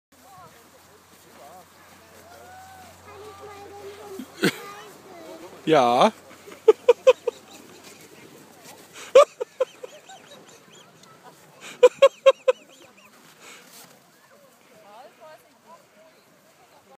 Schlittenfahrt - LOL
.nd ich sag noch: Schatz! Du musst hinten sitzen und unsere Kleine vorne.